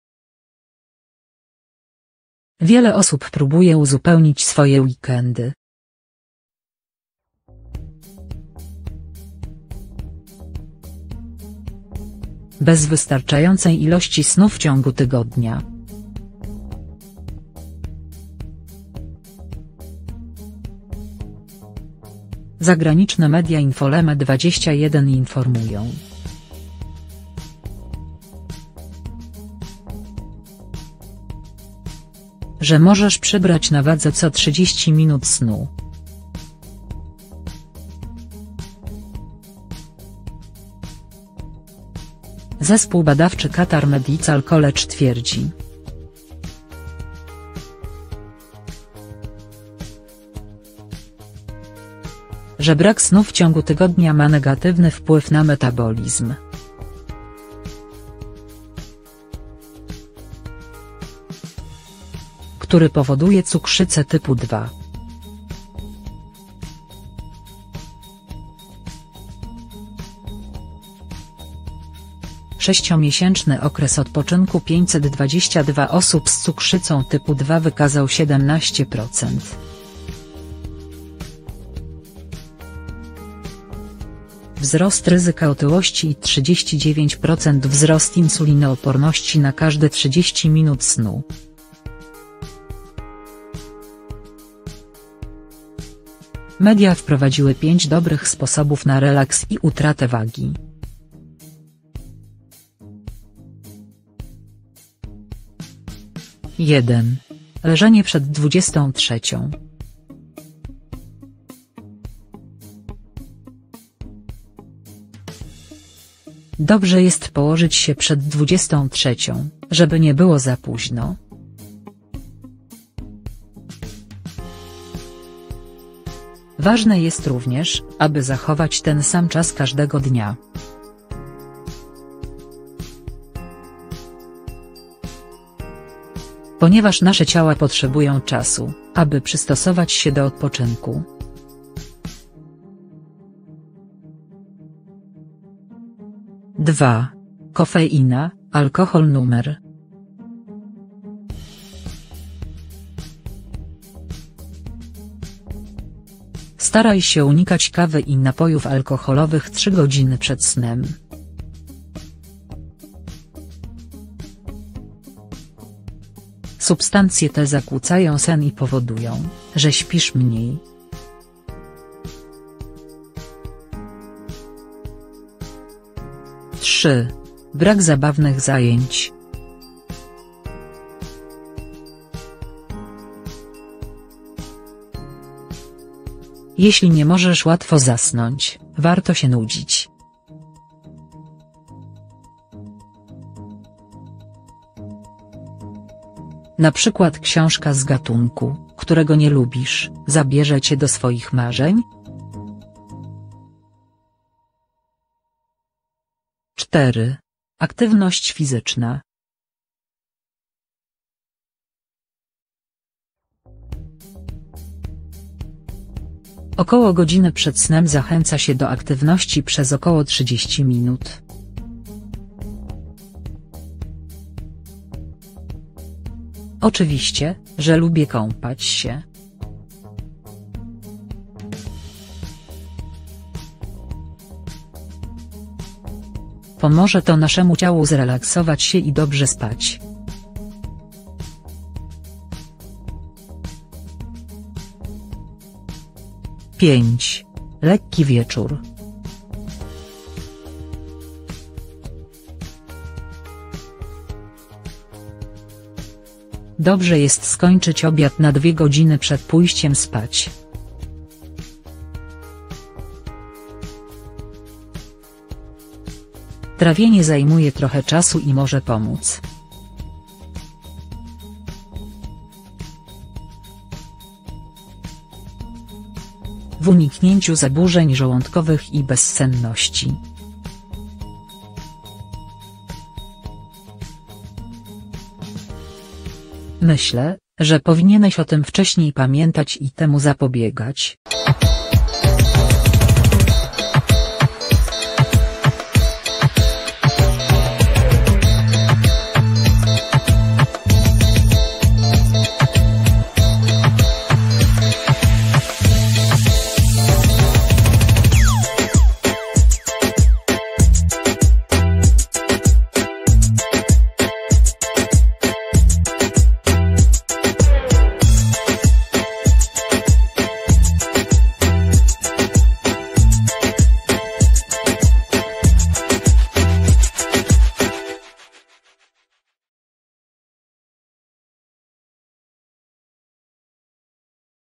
Co 30 minut snu zmniejsza twoje „ryzyko tłuszczu”
Wiele osób próbuje uzupełnić swoje weekendy bez wystarczającej ilości snu w ciągu tygodnia.

Zagraniczne media Infolema 21 informują, że możesz przybrać na wadze co 30 minut snu.